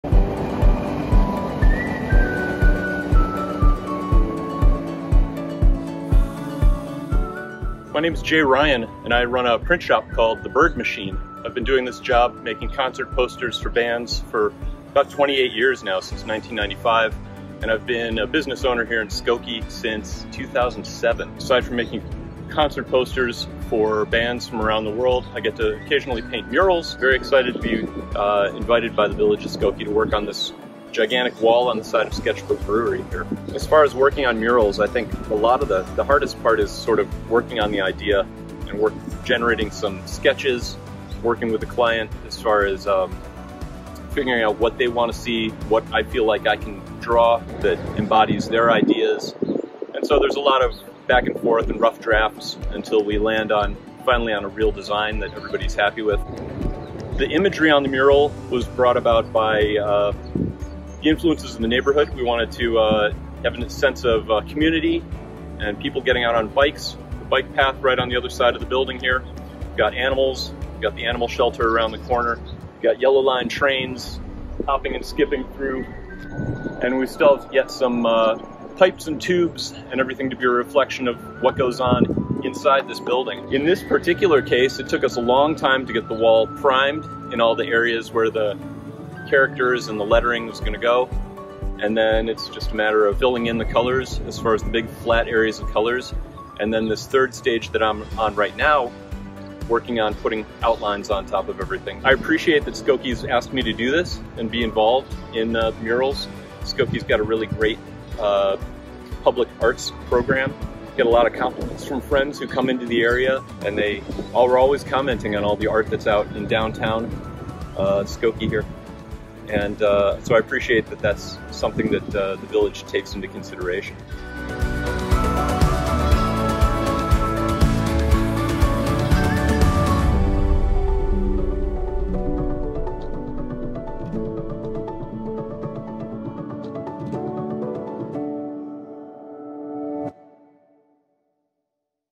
New Mural at Sketchbook Skokie!
Made possible by the Village of Skokie Public Arts Advisory Commission, this new mural by artist Jay Ryan of The Bird Machine brings color and whimsy to the Sketchbook Brewery space and the entire Skokie community!

Don't miss the November 5 , 2023 Art in the Alley celebration!